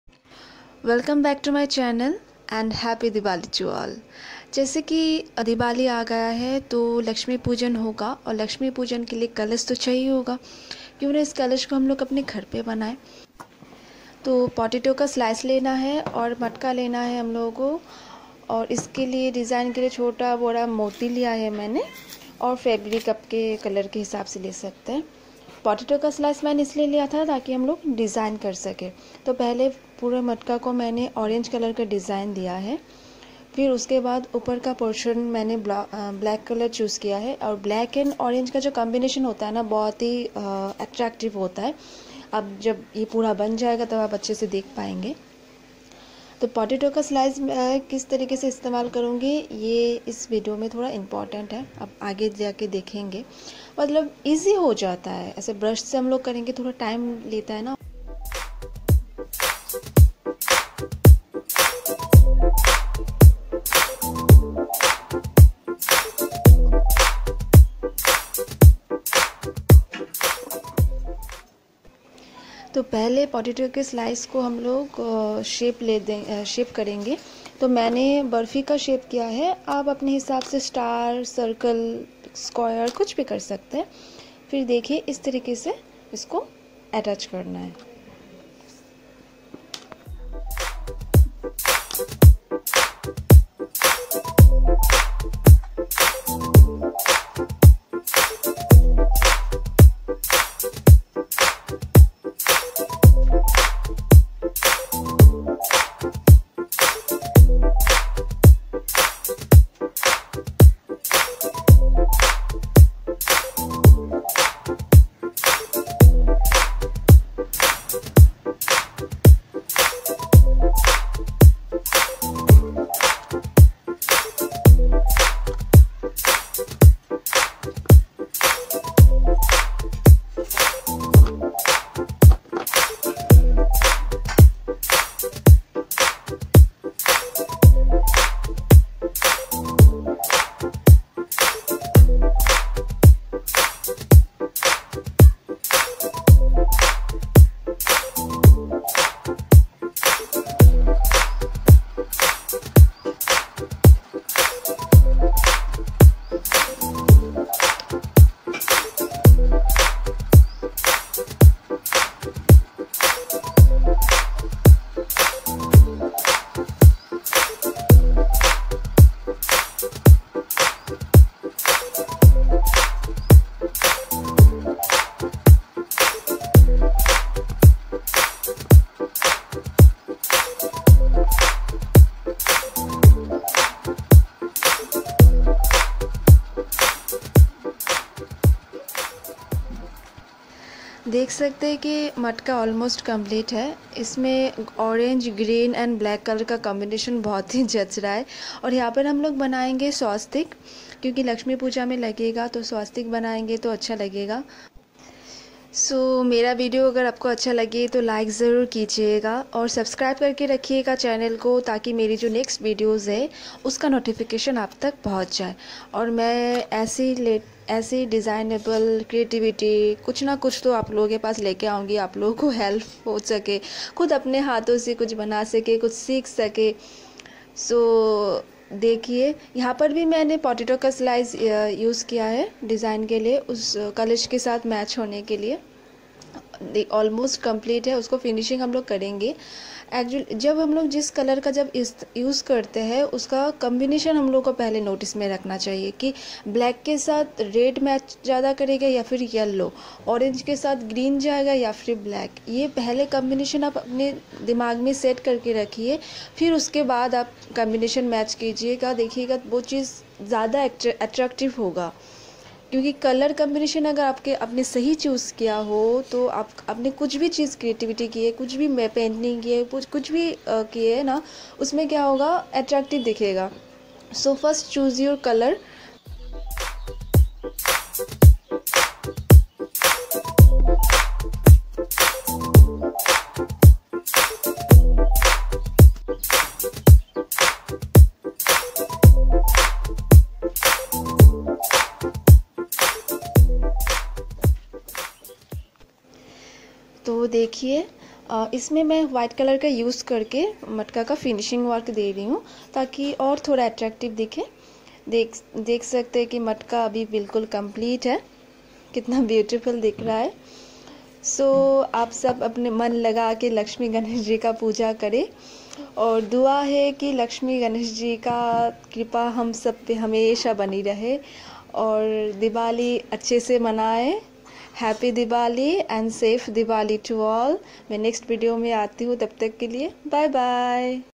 How to decorate kalash for Laxmi pooja/Navratri 🙏
Decorate kalash for laxmi / Ganesh ji pooja.
Materials you need only -
Kalash or matka
Fabric color - Orange , Black , Green , golden
Pearl white

Happy and Safe Diwali to all 💞

My previous videos link are as follows -

                     "Saireety Creation"

@ Itspiku_official27
@ saireety_creation9